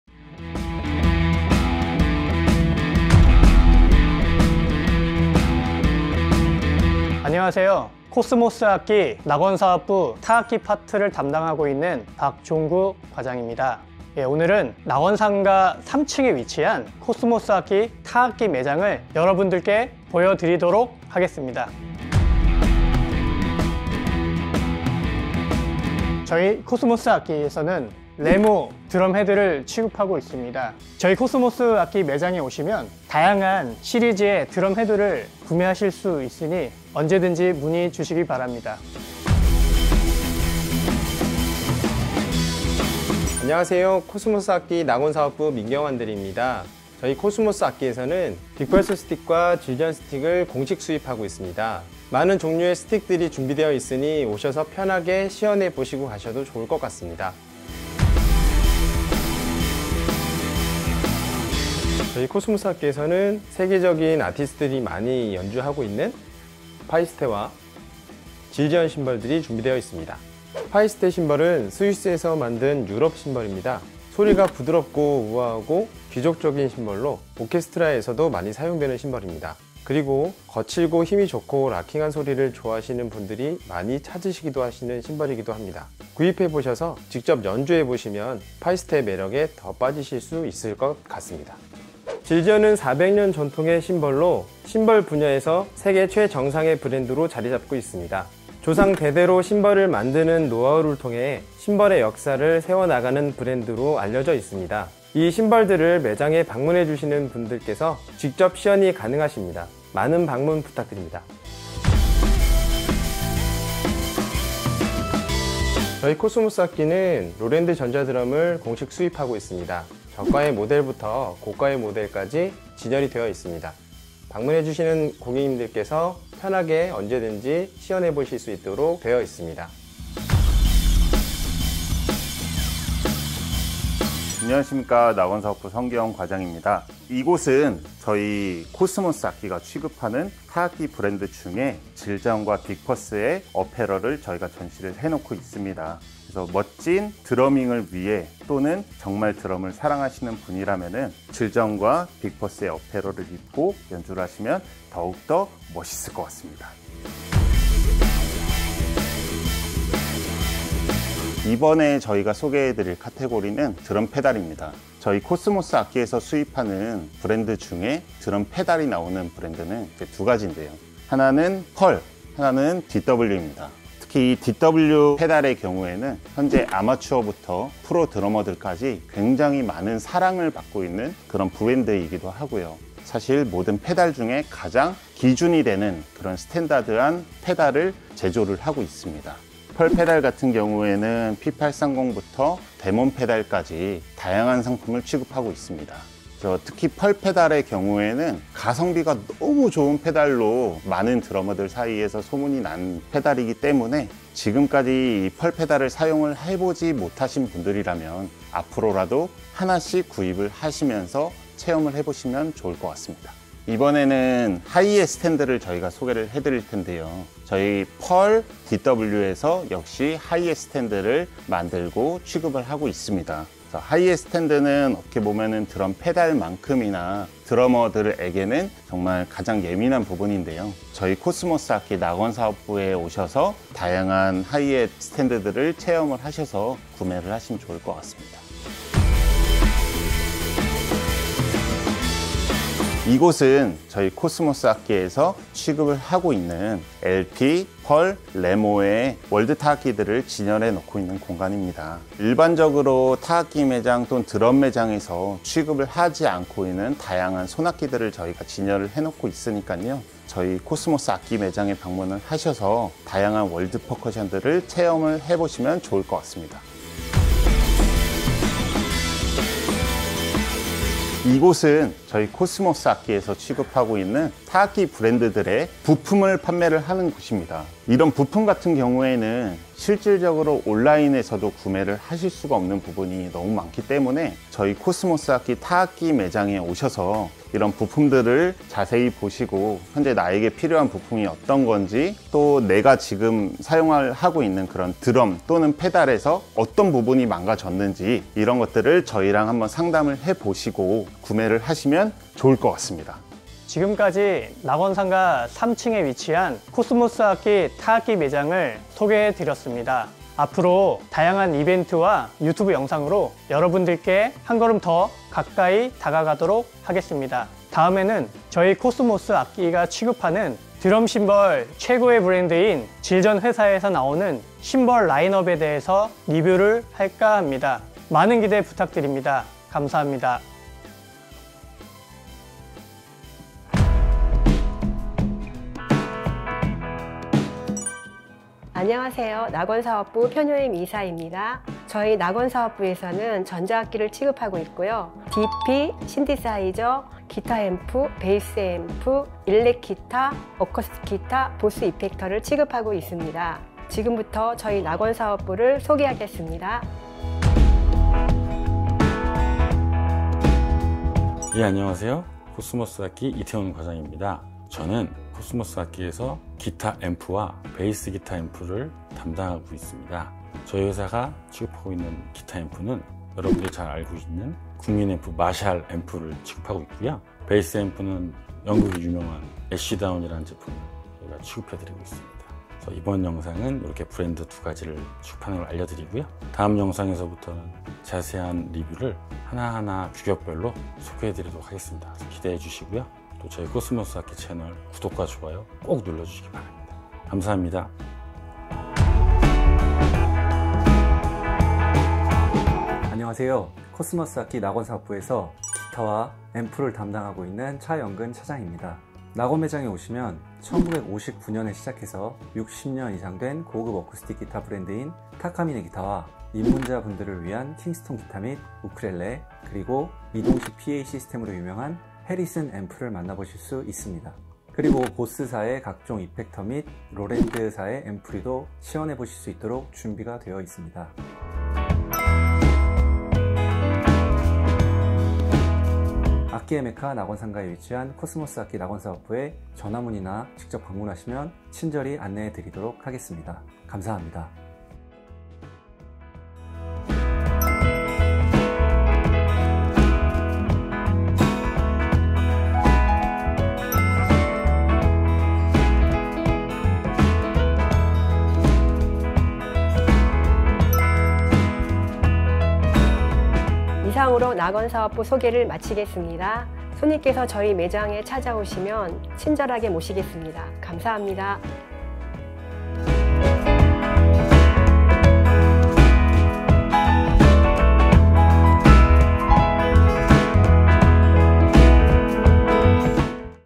낙원 상가의 랜드마크?! 낙원 상가 방문 필수 코스✨ 코스모스악기 낙원사업부 2F & 3F
낙원 상가에 방문한 적 있다면?
누구나 한 번쯤 본 적 있을 바로 그곳!
코스모스악기 낙원사업부를 소개합니다📢📢

⚪코스모스악기 낙원사업부 2F⚪
디지털 피아노, 신시사이저, 일렉기타 & 베이스 기타 & 어쿠스틱 기타,
앰프, BOSS 이펙터, 액세서리 등 취급

⚪코스모스악기 낙원사업부 3F⚪
PEARL, ZILDJIAN, REMO, DW, LP 취급 드럼 매장

악기의 메카, 코스모스악기 낙원사업부에 언제든지 편하게 방문해주세요😄